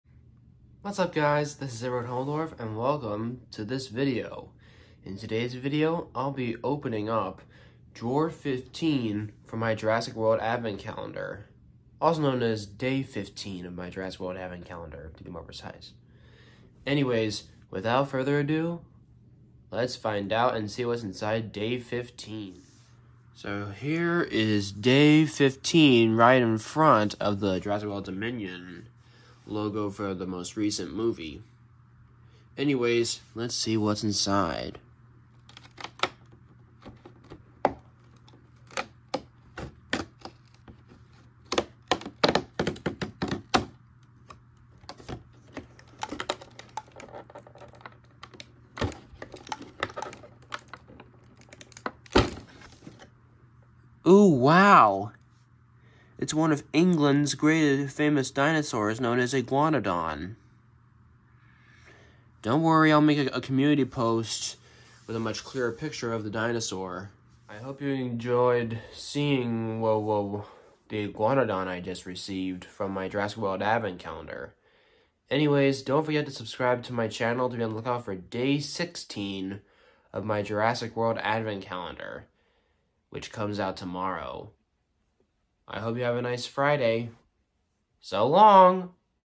Day 15 of my Jurassic World Advent Calendar
I received one of the dinosaurs seen in the prologue of Jurassic World Dominion before the T. Rex and Giganotosaurus battle. The prologue is on the Jurassic World YouTube Channel.